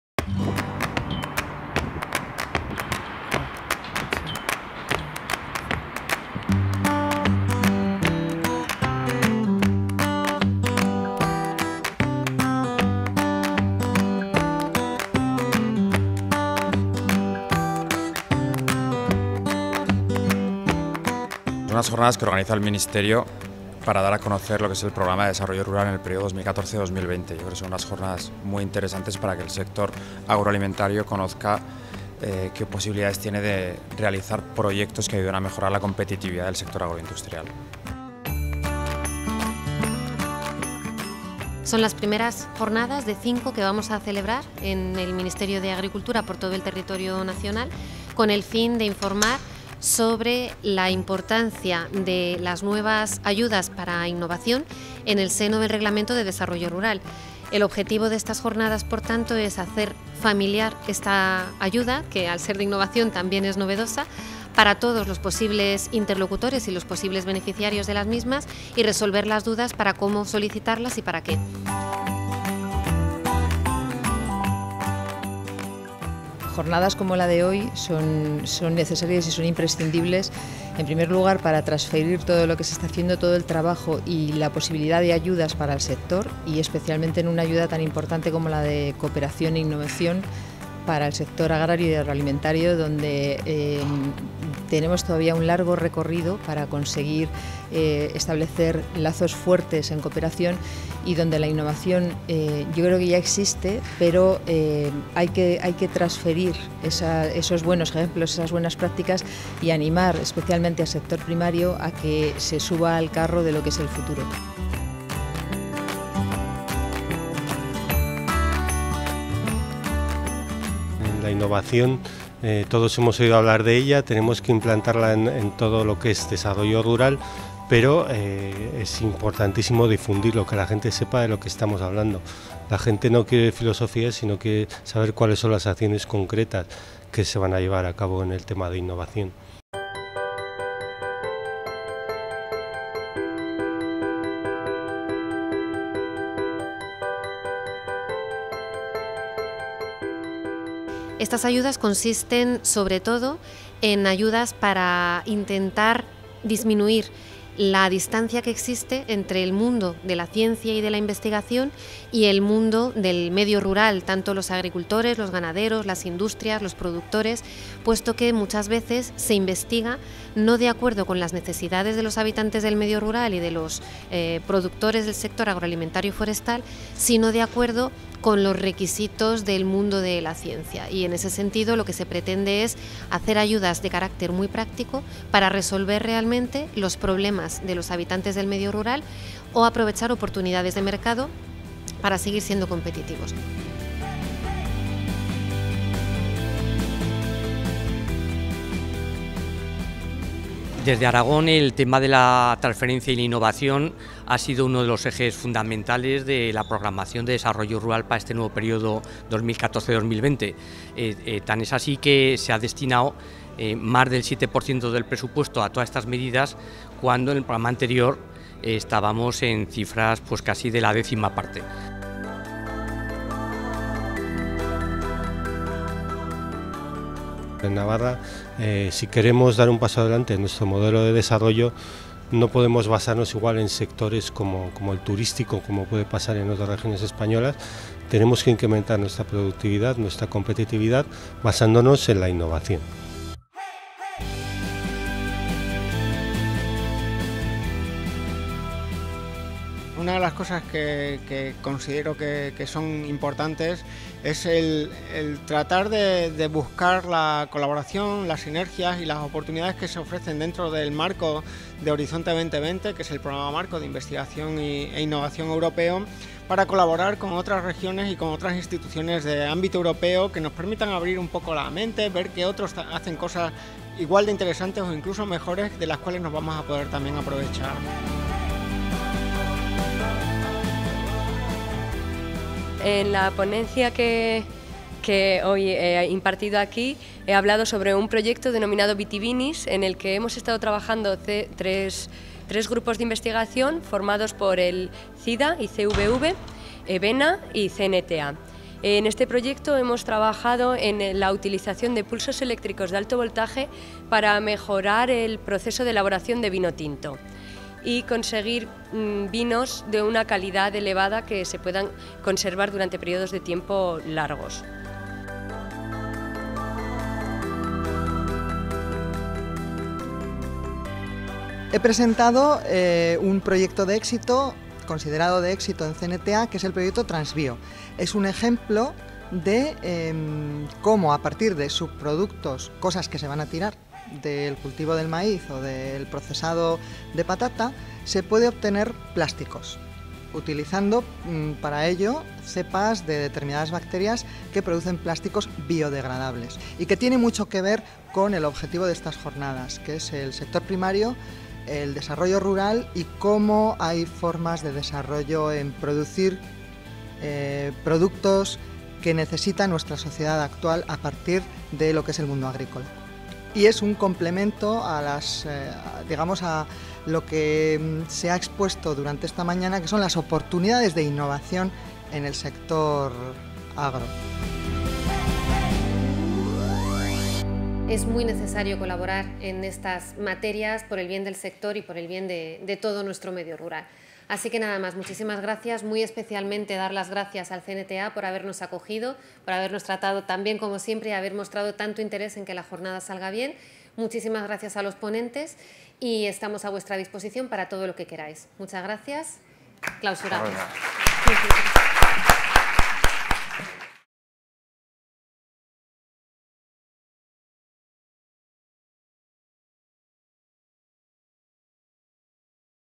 Primera jornada itinerante de innovación en el medio rural de la Red Rural Nacional
La Red Rural Nacional (RRN) tiene como tarea, entre las actividades integradas en su plan de acción, la organización de una serie de jornadas itinerantes sobre innovación a lo largo de este primer semestre de 2015. El día 18 de febrero de 2015 se celebró la primera de ellas, dirigida a los territorios de La Rioja, Aragón y Navarra, en el Centro Nacional de Tecnología y Seguridad Alimentaria (CNTA) de San Adrián, Navarra, en la que participaron más de 70 personas, entre Autoridades de Gestión de dichas CC.AA. y potenciales beneficiarios de la medida de innovación.